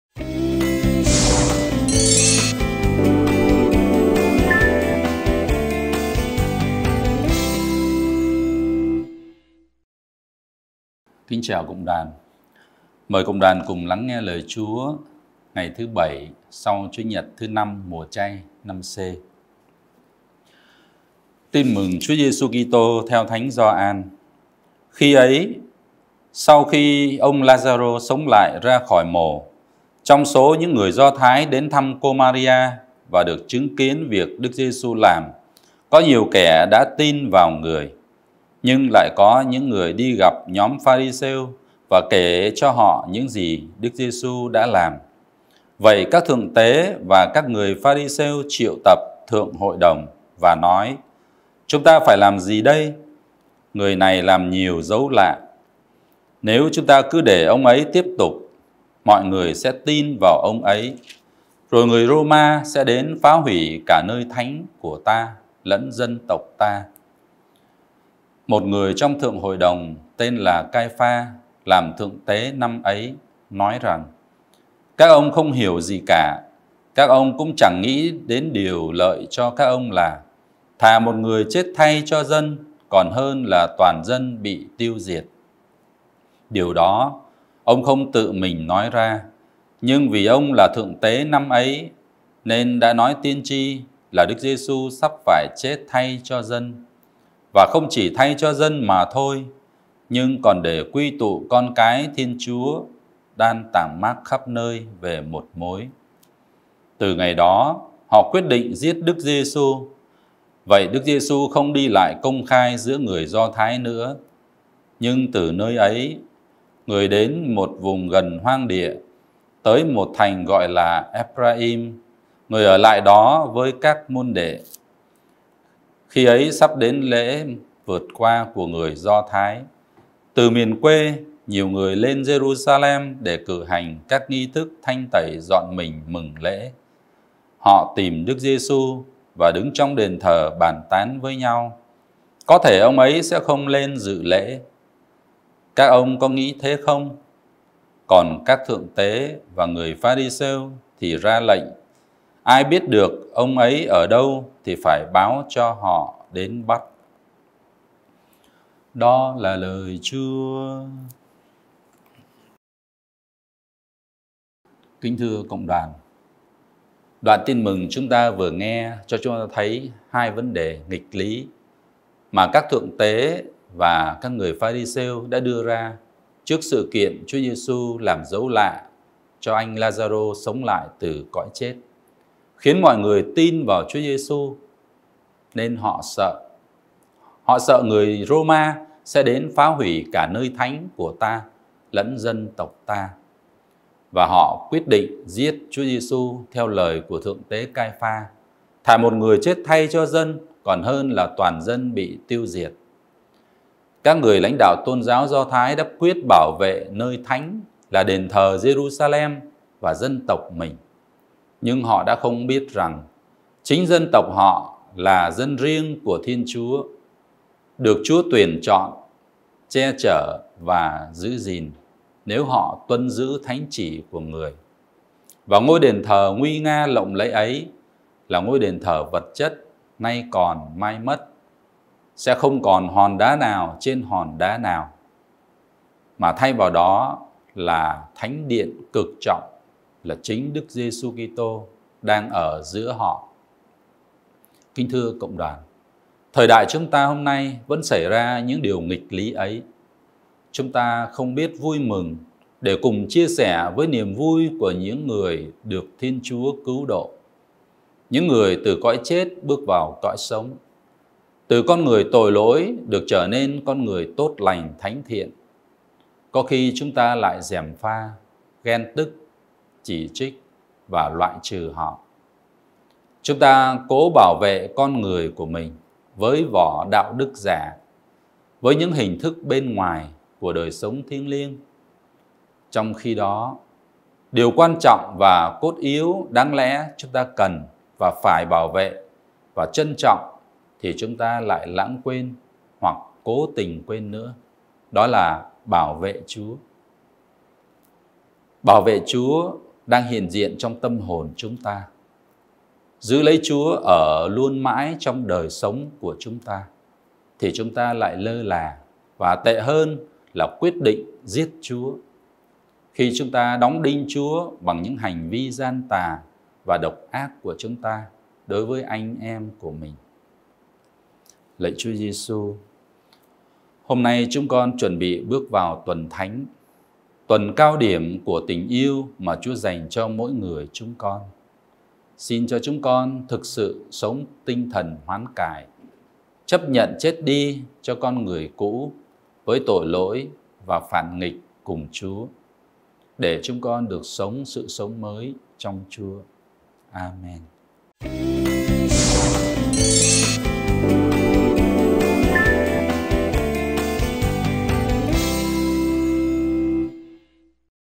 Lời Chúa - Thứ Bảy tuần V mùa Chay 09/04/2022
Suy niệm Lời Chúa - Thứ Bảy tuần V mùa Chay 09/04/2022 - Lm. Gioan Baotixita Lê Trung Ân, C.Ss.R.